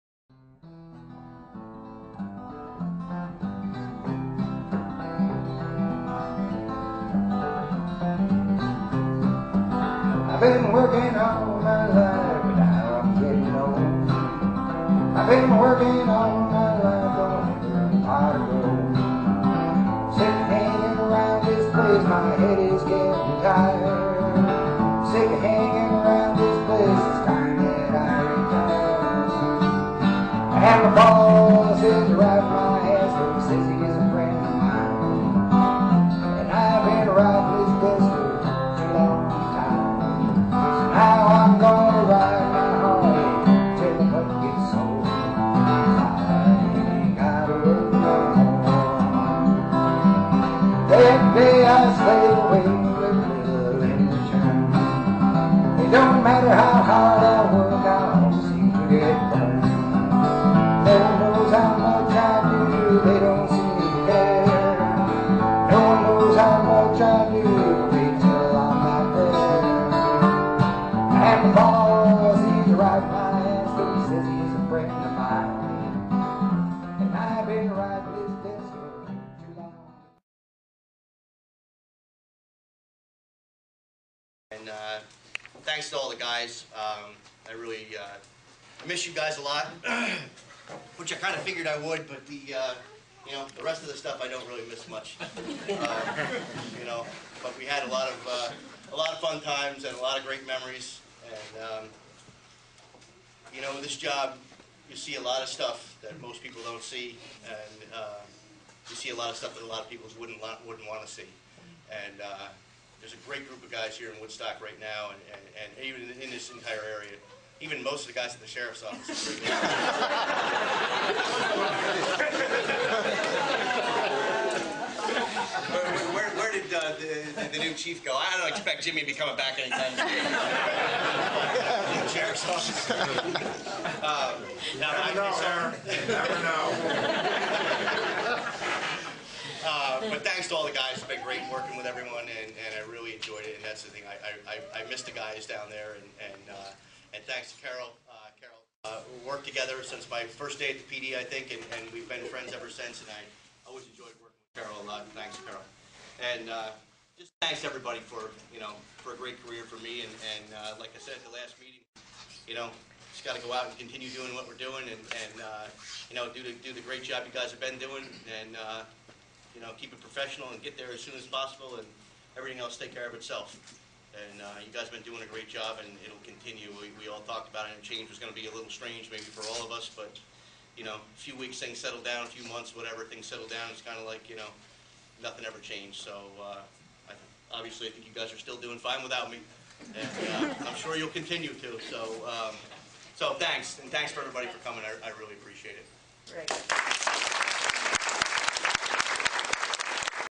Harry Baldwin's retirement party pics
pic of the guest speakers and the guests